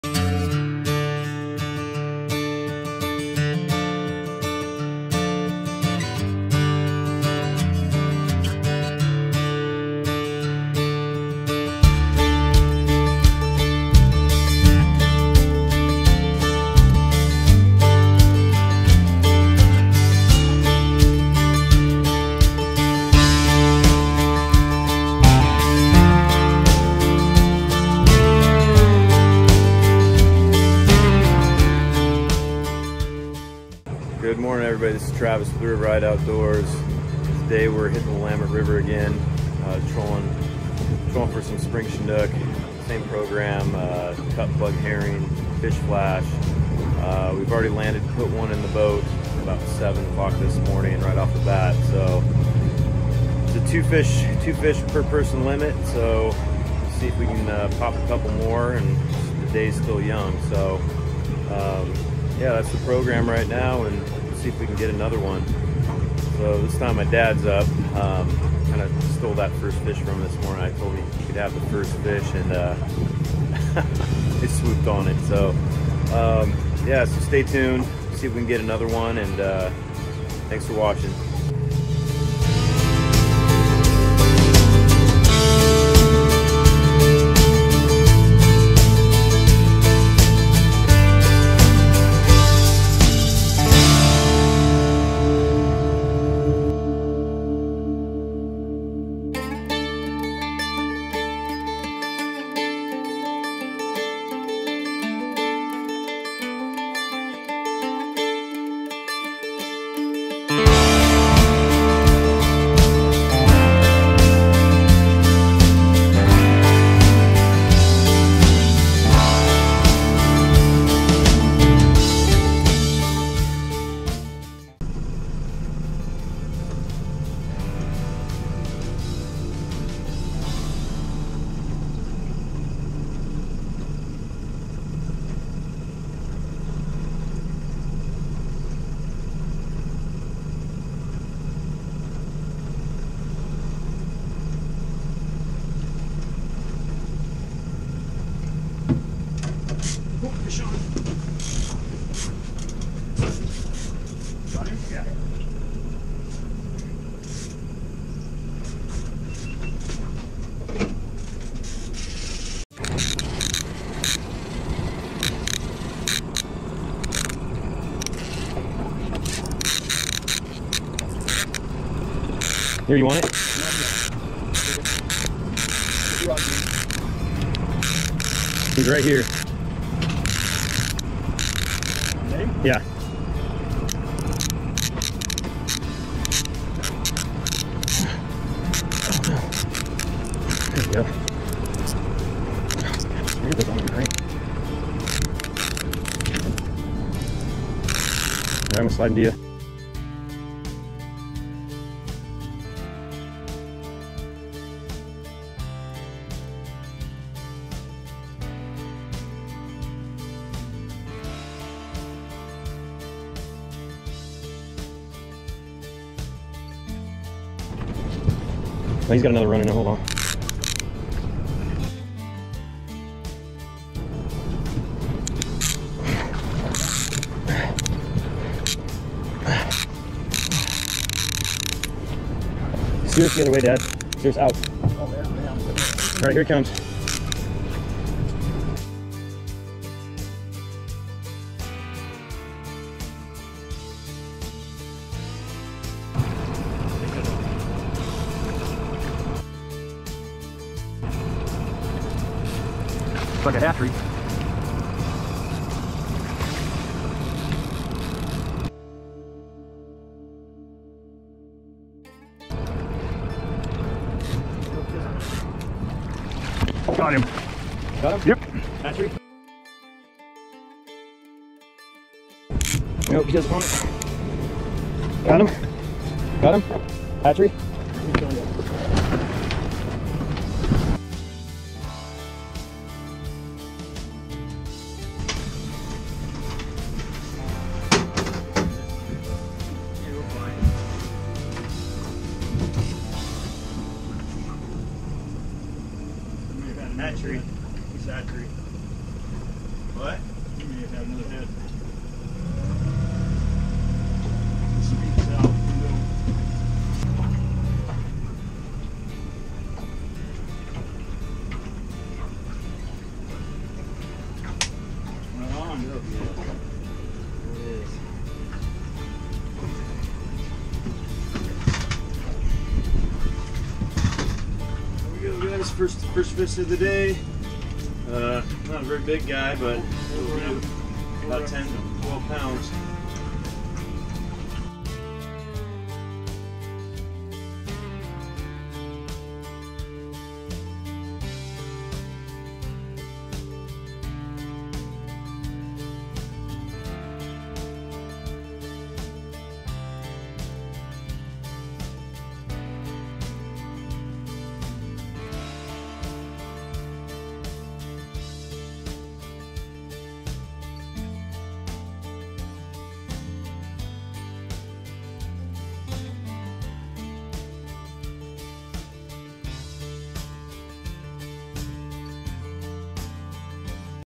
Willamette River Spring Chinook TAKEDOWNS // Line RIPPING Off Reel
In this video we are back out searching for spring chinook. Fish flash and herring were still on the menu to catch these chinook. Spring chinook season has still been hit and miss. Our first pass of the morning turned out to be our only chance for the day. We hooked the 12lb springer at 7am in the morning as the sun was coming up. Spring chinook fishing should start picking up here in May and June until then we will still keep scratch one here and one there . Good luck to everyone chasing these amazing spring chinook.

Enjoy and thanks for checking it out .

More videos to come cheers!

ROD AND REEL
1st ROD
2nd ROD
FISH FLASH

NET
BECKMAN NET

CAMERA GEAR
GO PRO
DJI OS 4
ND FILTERS
Clamp mount